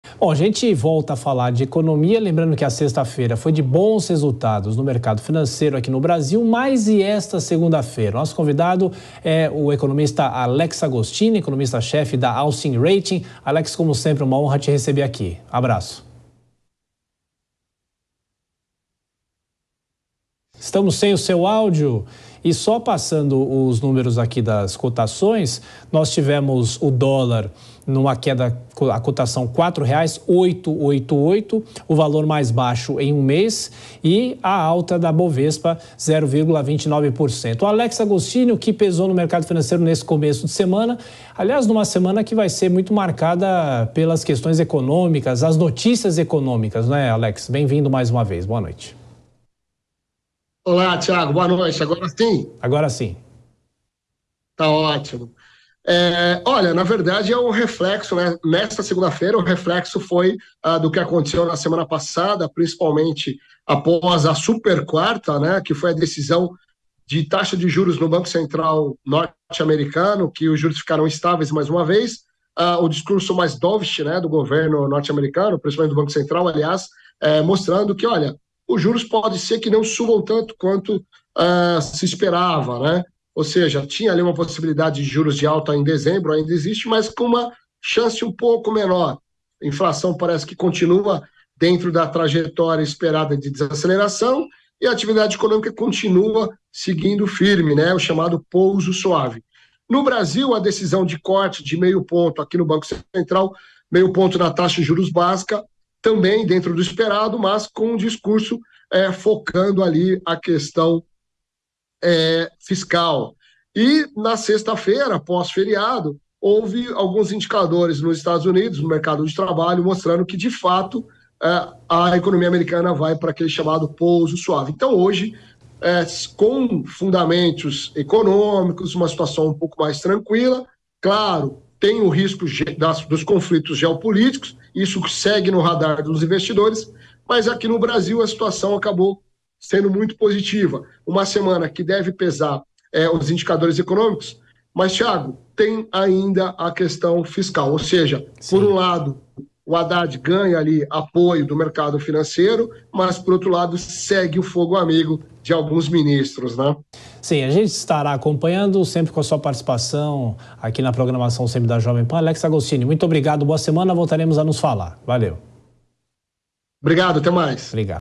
Ibovespa fecha em alta pelo quarto pregão consecutivo; Alex Agostini analisa
O Ibovespa fechou o pregão desta segunda (06) com o quarto dia de alta consecutiva, fechando o dia em 0,23%.  Na semana passada, a alta acumulada da Bolsa chegou a 4%. O dólar permaneceu estável, sendo negociado a R$ 4,89. O economista Alex Agostini analisou as movimentações do mercado financeiro.